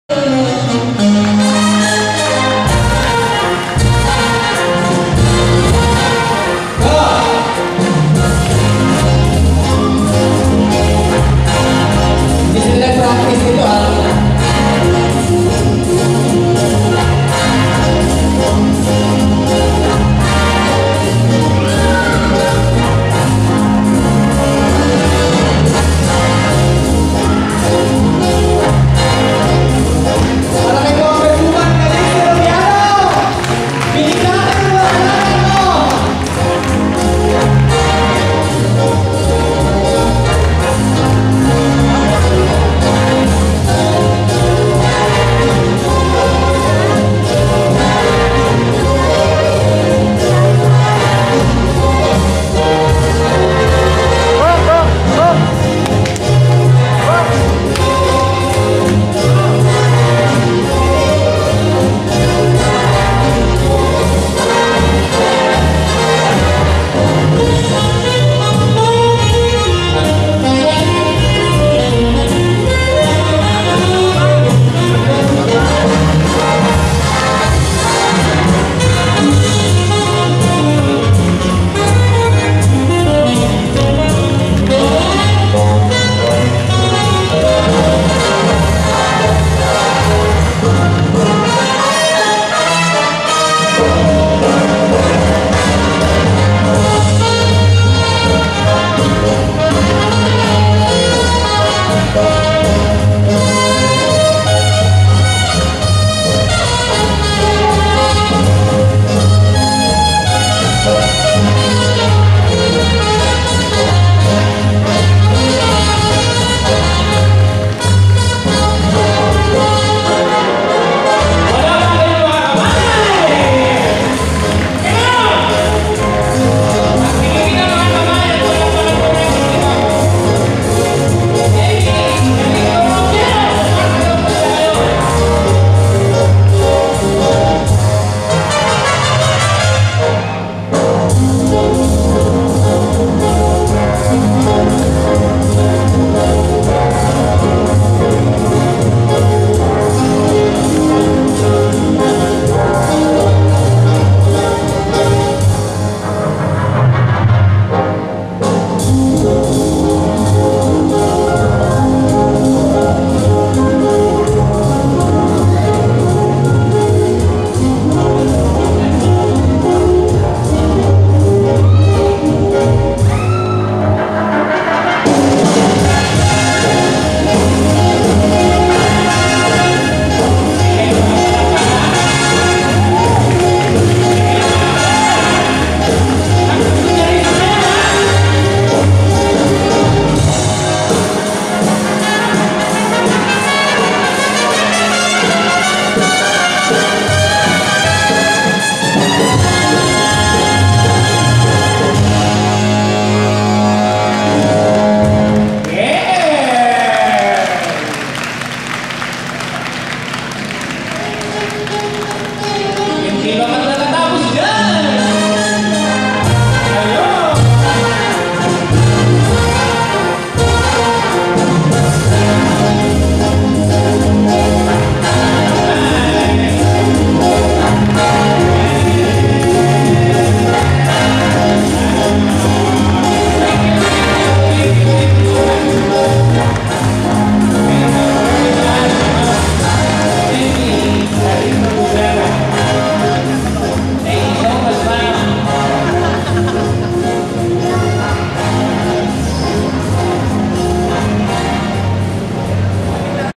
Dancing Emi Calixto w/ BMG Team pasay
Promoting Dance Sports In pasay city in Partnership w/ Deped Teachers